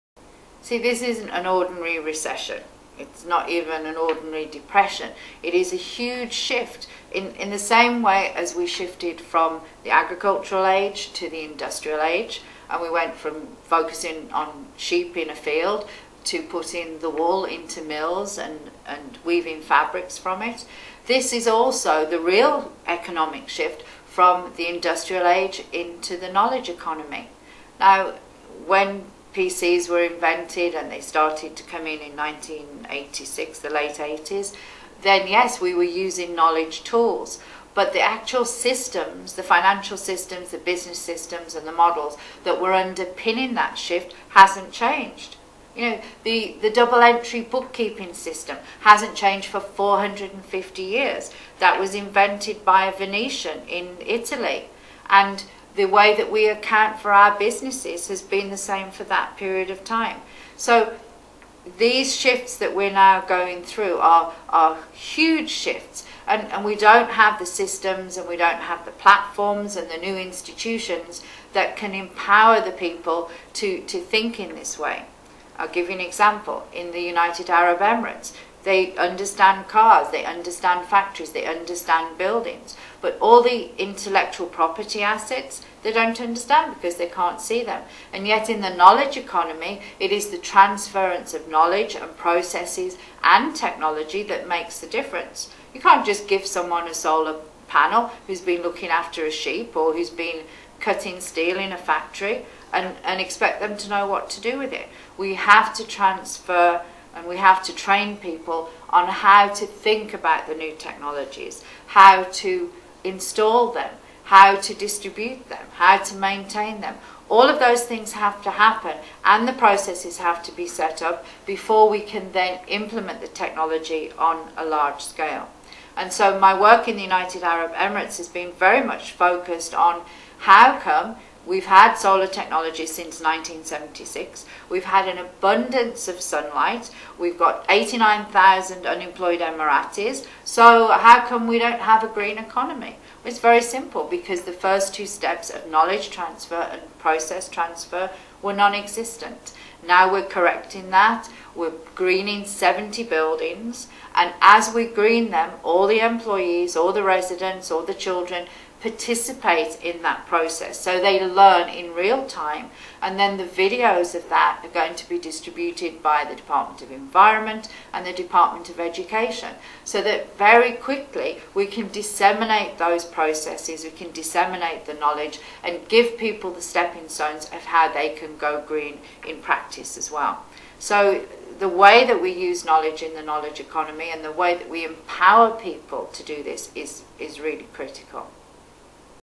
Massive Shift from the Industrial Age to the Knowledge Economy
The transfer of knowledge, processes and technologies are key to emerging a green economy.